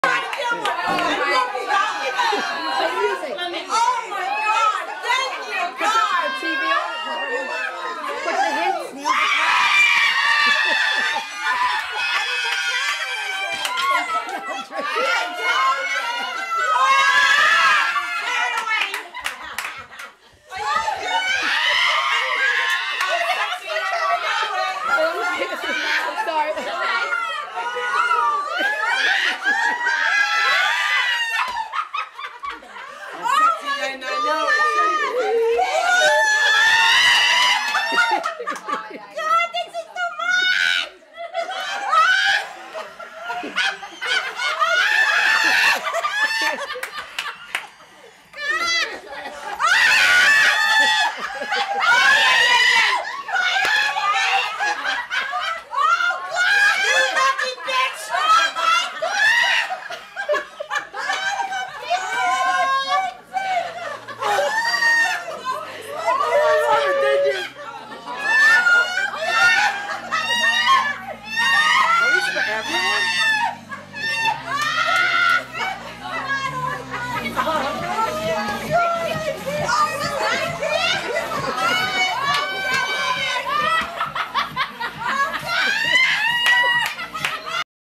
Surprise stripper‼️
She thought she was really getting arrested...ROTFLMFAO!